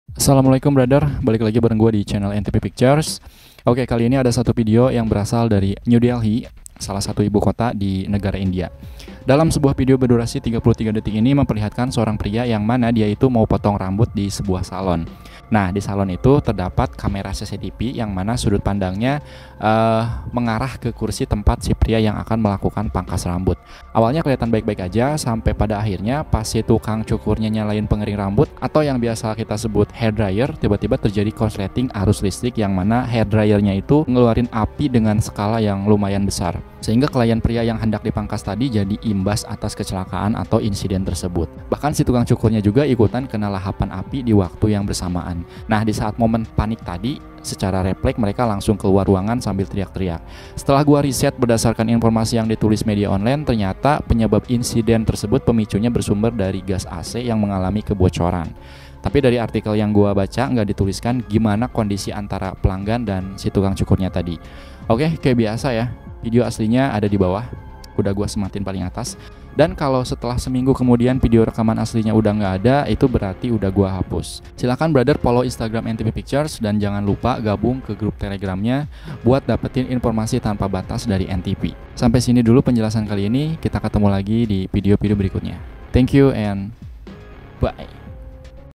AMPIR DIPANGKAS SAMA KELAPANYA INI MAH | INSIDEN DI BARBERSHOP
𝐊𝐄𝐑𝐉𝐀𝐒𝐀𝐌𝐀 𝐁𝐈𝐒𝐍𝐈𝐒 📩